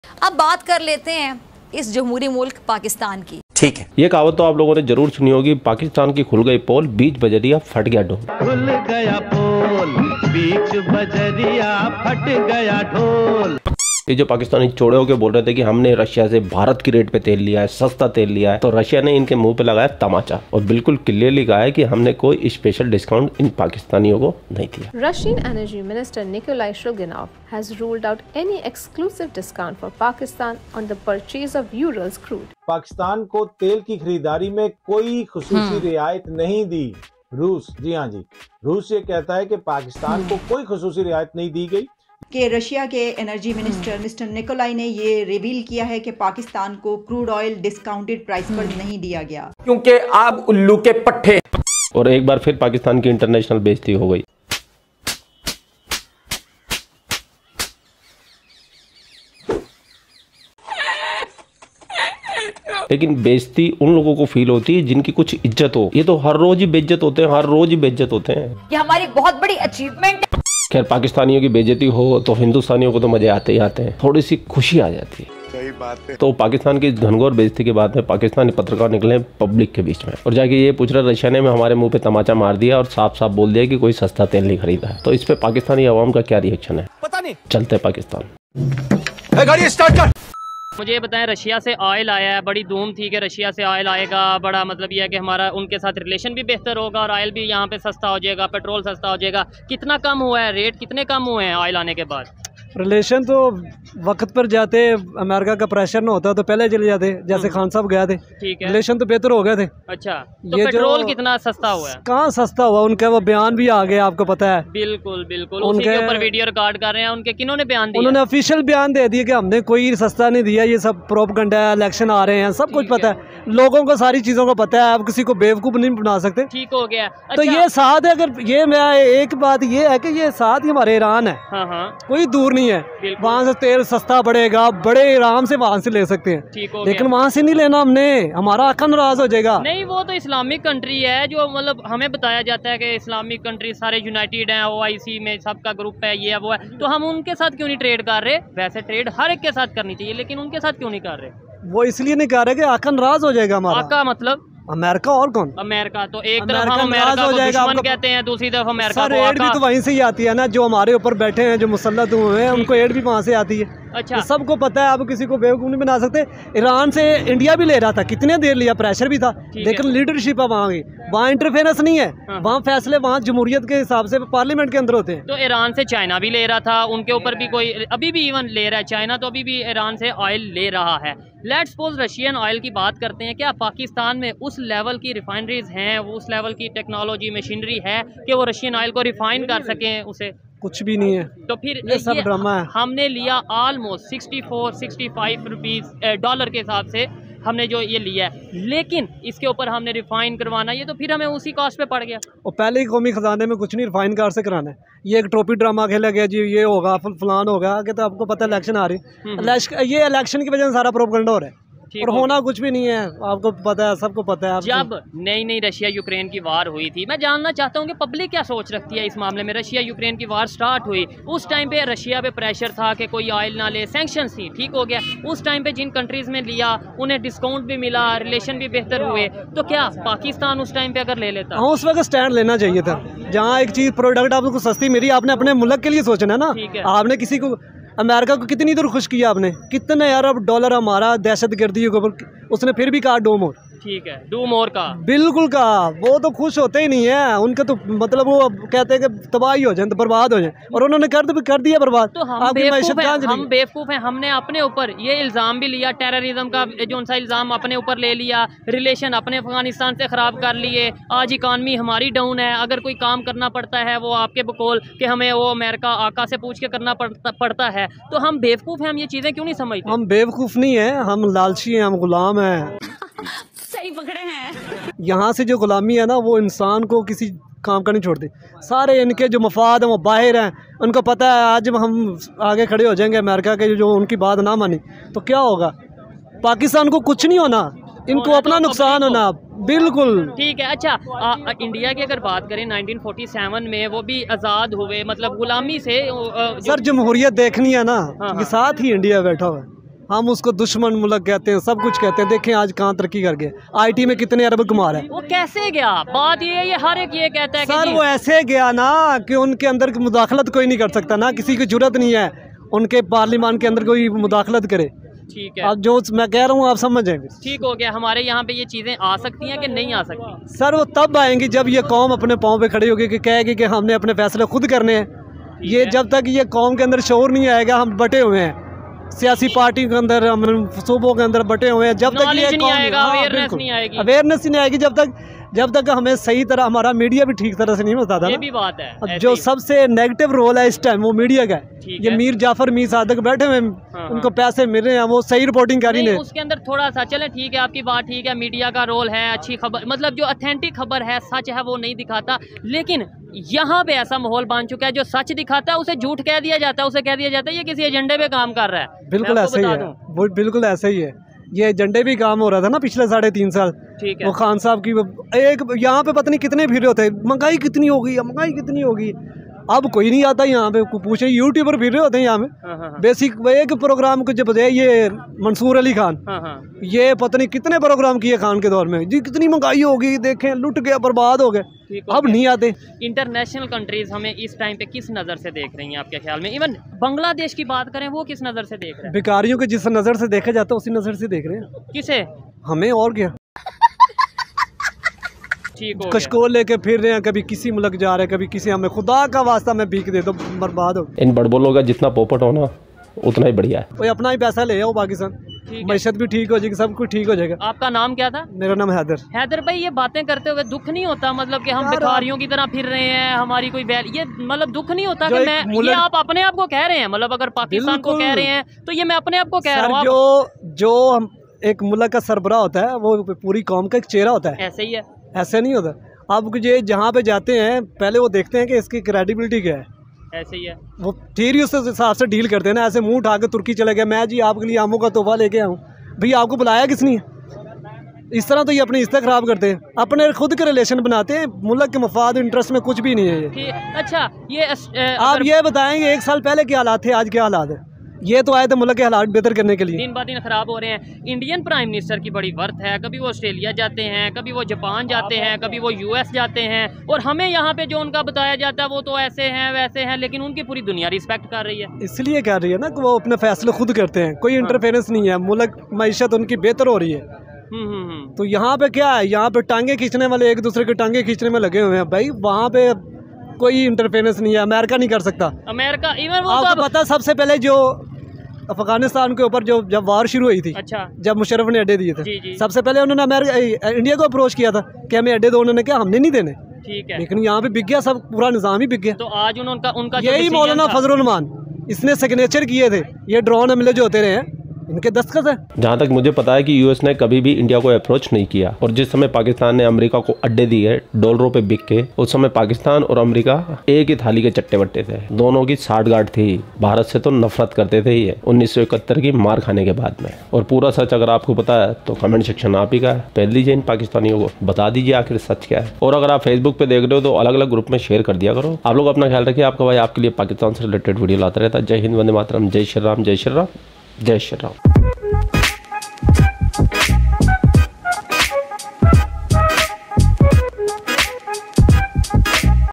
International humiliation of Pakistan once again 😀
International humiliation of Pakistan once again 😀 | Russia Says It's Selling Oil to Pakistan Without Special Discount | PAKISTANI FUNNY REACTION